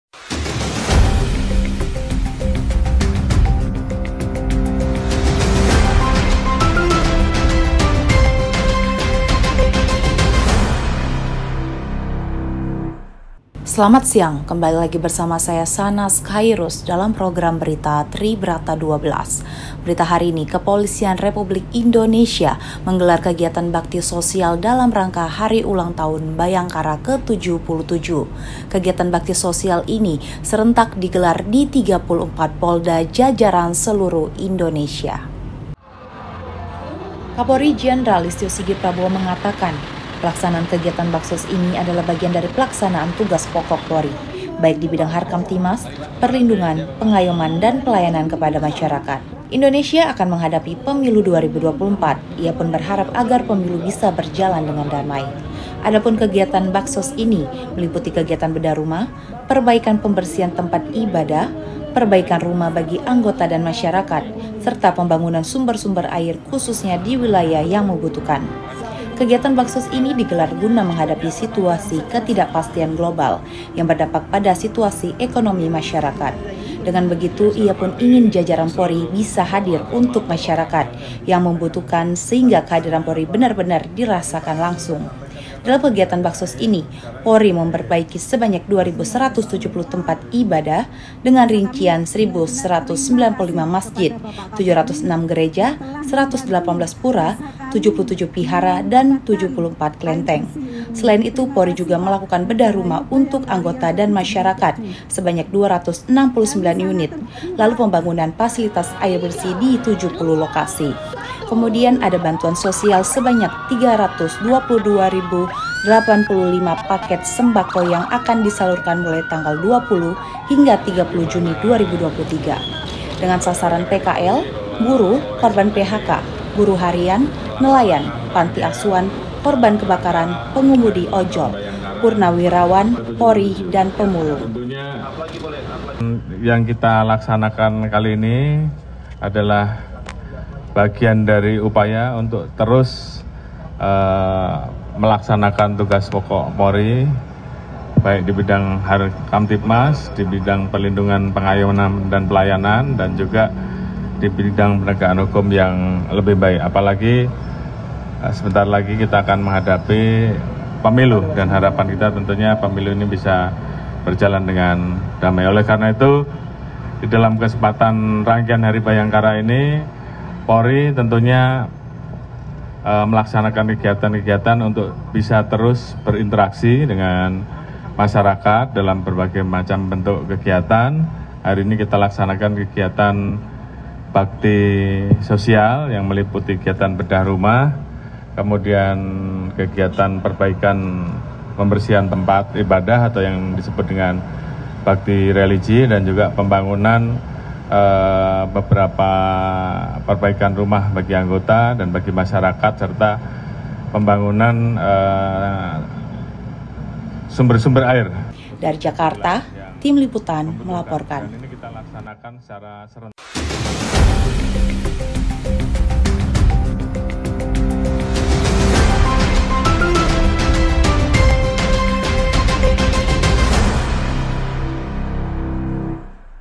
KAPOLRI GELAR BAKSOS DALAM RANGKA HARI BHAYANGKARA KE 77
KEPOLISIAN NEGARA REPUBLIK INDONESIA (POLRI) MENGGELAR KEGIATAN BAKTI SOSIAL (BAKSOS) DALAM RANGKA HUT BHAYANGKARA KE-77. PELAKSANAAN KEGIATAN BAKSOS INI SERENTAK DIGELAR DI 34 POLDA JAJARAN SELURUH INDONESIA.

KAPOLRI JENDERAL LISTYO SIGIT PRABOWO MENGATAKAN, PELAKSANAAN KEGIATAN BAKSOS INI ADALAH BAGIAN DARI PELAKSANAAN TUGAS POKOK POLRI, BAIK DI BIDANG HARKAMTIBMAS, PERLINDUNGAN, PENGAYOMAN DAN PELAYANAN KEPADA MASYARAKAT.

APALAGI, KATA KAPOLRI, INDONESIA AKAN MENGHADAPI PEMILU 2024. IA PUN BERHARAP AGAR PEMILU BISA BERJALAN DENGAN DAMAI.

ADAPUN KEGIATAN BAKSOS INI, MELIPUTI KEGIATAN BEDAH RUMAH, PERBAIKAN PEMBERSIHAN TEMPAT IBADAH ATAU BIASA DISEBUT BAKTI RELIGI, PERBAIKAN RUMAH BAGI ANGGOTA DAN MASYARAKAT, SERTA PEMBANGUNAN SUMBER-SUMBER AIR, KHUSUSNYA DI WILAYAH YANG MEMBUTUHKAN.

KEGIATAN BAKSOS INI DIGELAR GUNA MENGHADAPI SITUASI KETIDAKPASTIAN GLOBAL YANG BERDAMPAK PADA SITUASI EKONOMI MASYARAKAT.

DENGAN BEGITU, IA PUN INGIN JAJARAN POLRI BISA HADIR UNTUK MASYARAKAT YANG MEMBUTUHKAN, SEHINGGA KEHADIRAN POLRI BENAR-BENAR DIRASAKAN LANGSUNG.

DALAM KEGIATAN BAKSOS INI, POLRI MEMPERBAIKI SEBANYAK 2.170 TEMPAT IBADAH DENGAN RINCIAN 1.195 MASJID, 706 GEREJA, 118 PURA, 77 VIHARA, DAN 74 KLENTENG.

SELAIN ITU, POLRI JUGA MELAKUKAN BEDAH RUMAH UNTUK ANGGOTA DAN MASYARAKAT SEBANYAK 269 UNIT. LALU PEMBANGUNAN FASILITAS AIR BERSIH DI 70 LOKASI.

KEMUDIAN ADA BANTUAN SOSIAL SEBANYAK 322.085 PAKET SEMBAKO YANG AKAN DISALURKAN MULAI TANGGAL 20 HINGGA 30 JUNI 2023, DENGAN SASARAN PKL, BURUH KORBAN PHK, BURUH HARIAN, NELAYAN, PANTI ASUHAN, KORBAN KEBAKARAN, PENGEMUDI OJOL, PURNAWIRAWAN POLRI DAN PEMULUNG.